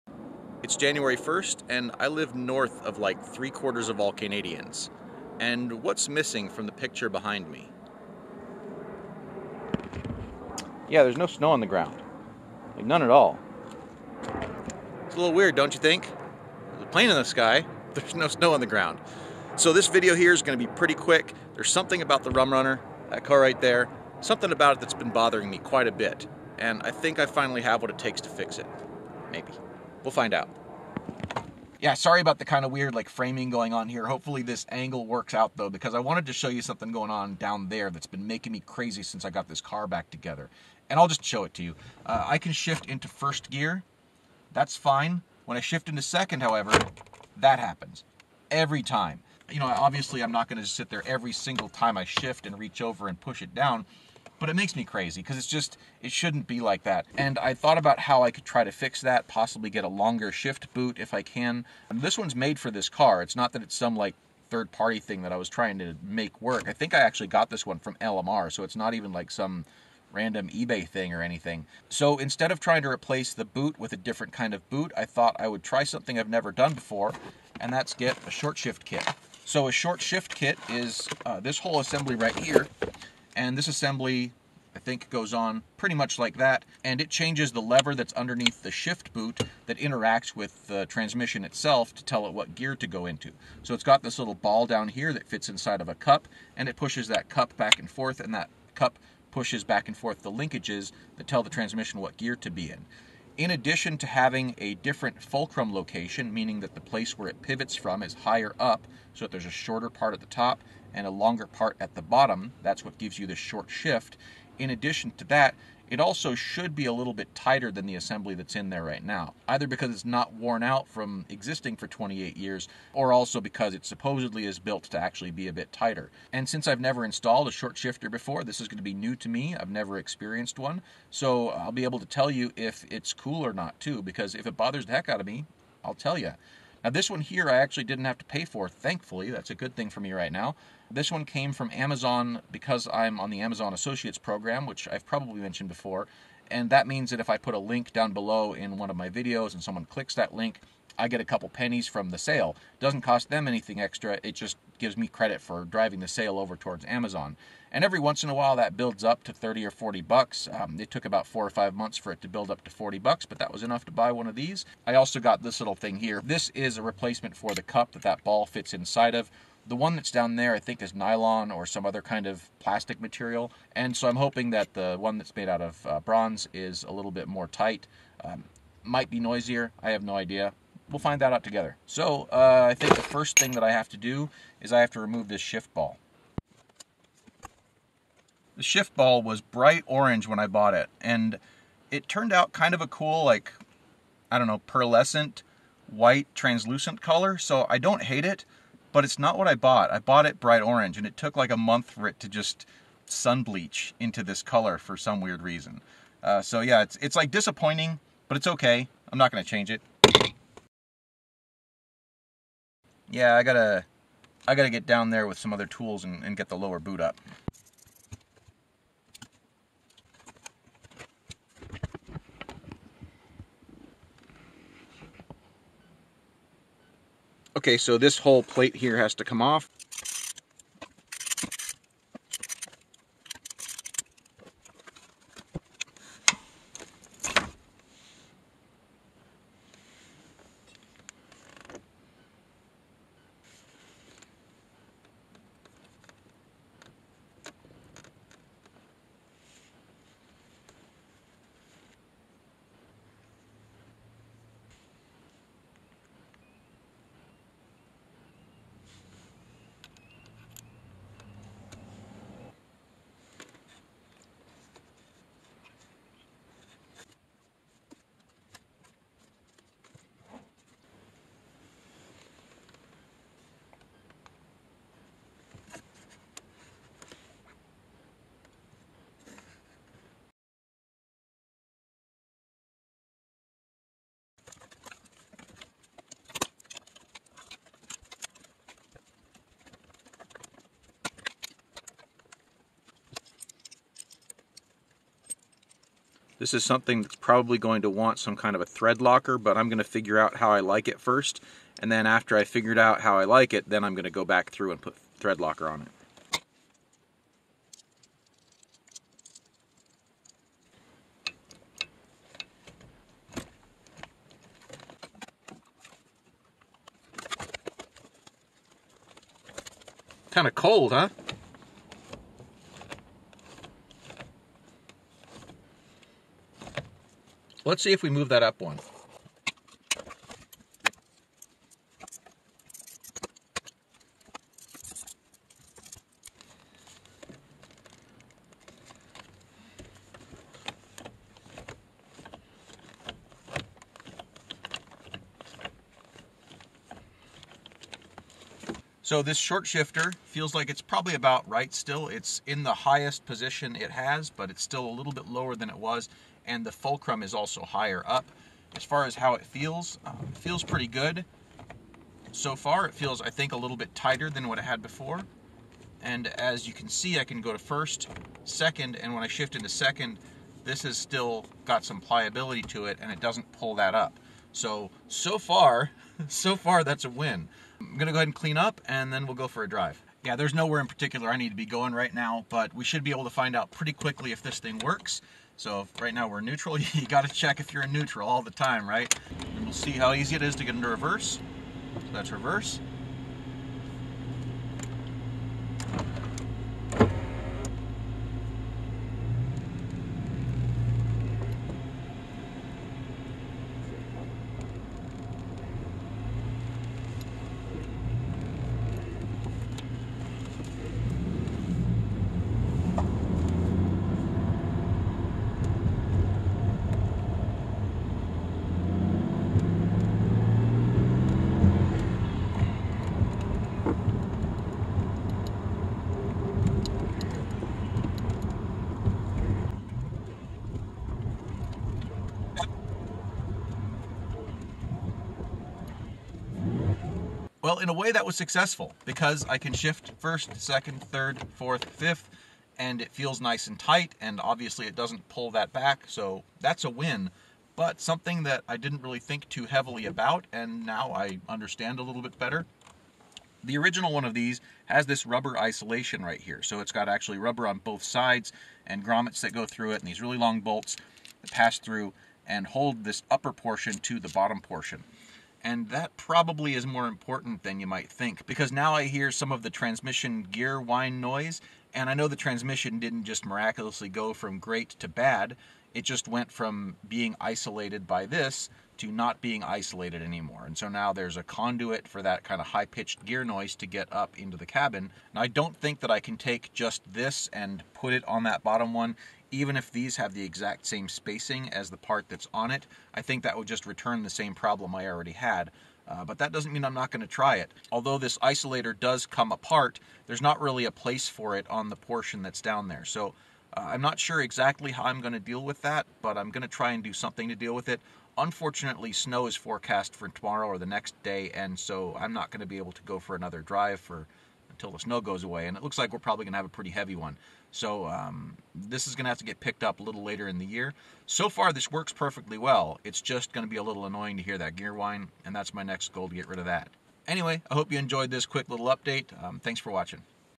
Short Shifter Install - Save A Stang 39
This quick update just addresses a minor nuisance that occurs when shifting into 2nd or 4th gear,  and talks about what's right and wrong with a short shift kit.